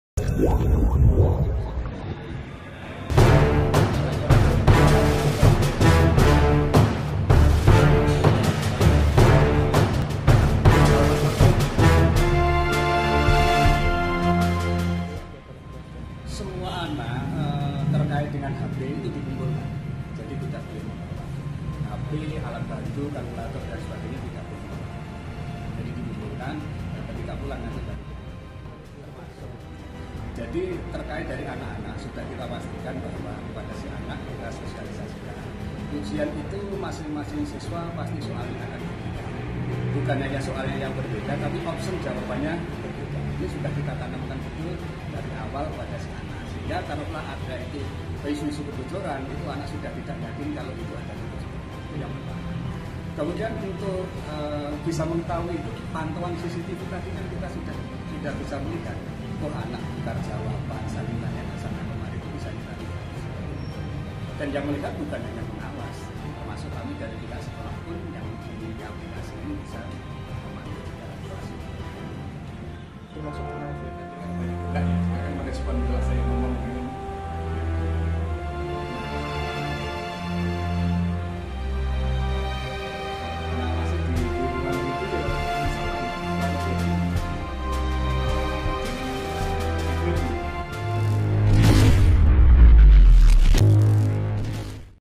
Smk Negeri 3 Membantah Beredarnya Kunci Jawaban Unbk
Smk negeri 3 telah menerapkan sistem keamanan berlapis sebelum UNBK diimulai, komputer dicek oleh proktor dan teknisi.

Video Journalist : Fahrizal Tito
Editor : Reyhan Sinatria